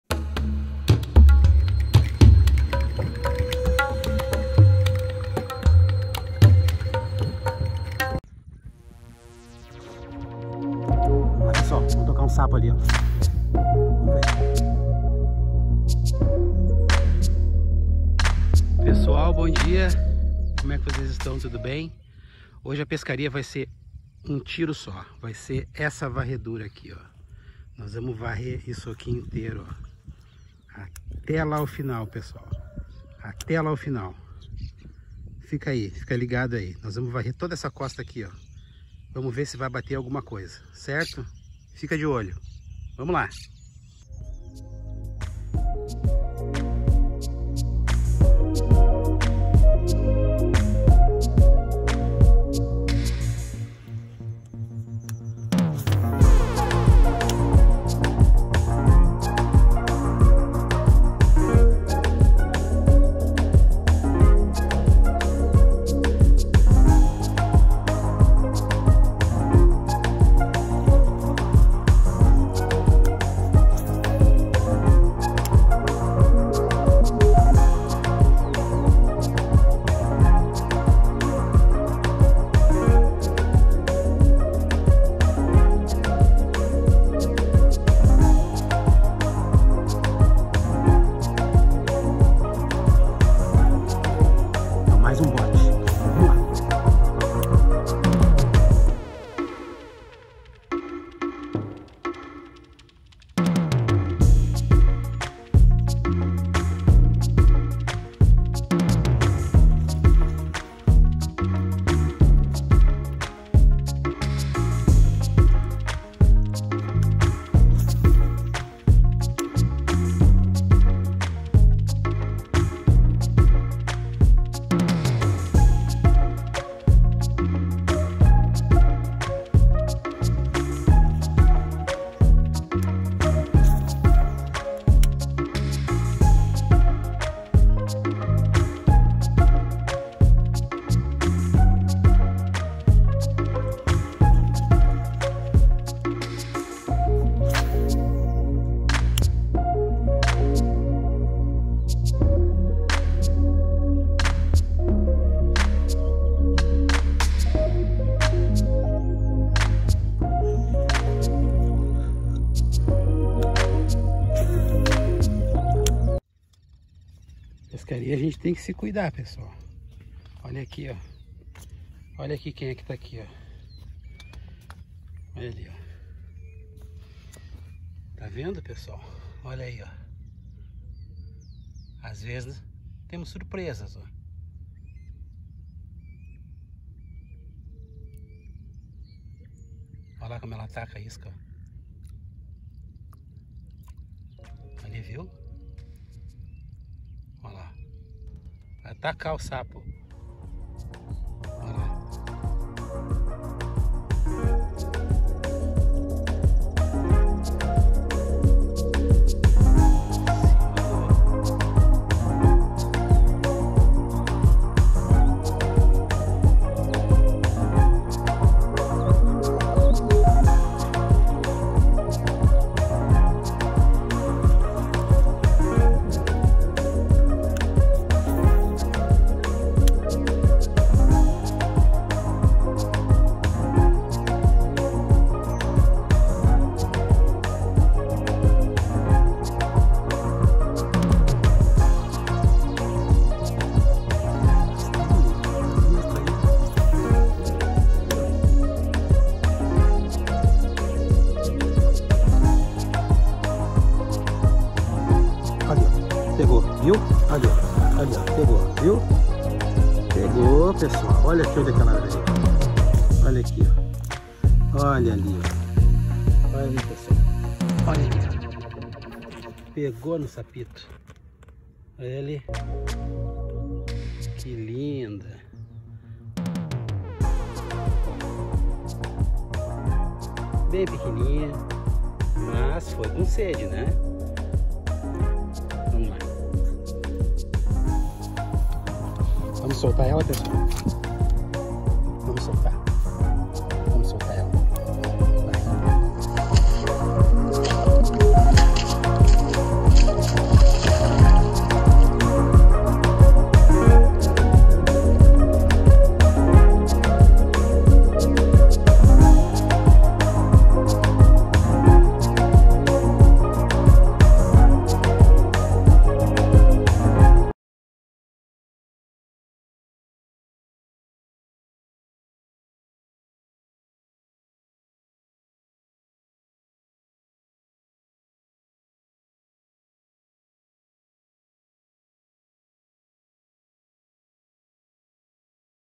COBRA - PERIGO NA PESCARIA DE TRAÍRA PESCARIA ROOT FISHING BRAZIL
COBRA - PERIGO NA PESCARIA PESCARIA ROOT FISHING BRAZIL

Fomos pescar e no caminho uma surpresa.
To cuidado é pouco numa pescaria. Desta vez nenhuma intercorrência, mas sempre é bom estar alerta.

Vamos pescar!